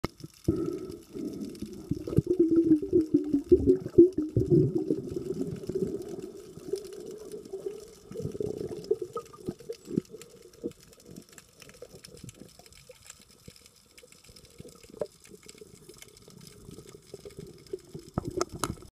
Bait Fish at Navarre Beach Marine Sanctuary and Artificial Reef
Shot with an iPhone 5S using Watershot housing at Navarre Beach Marine Sanctuary and Artificial Reef.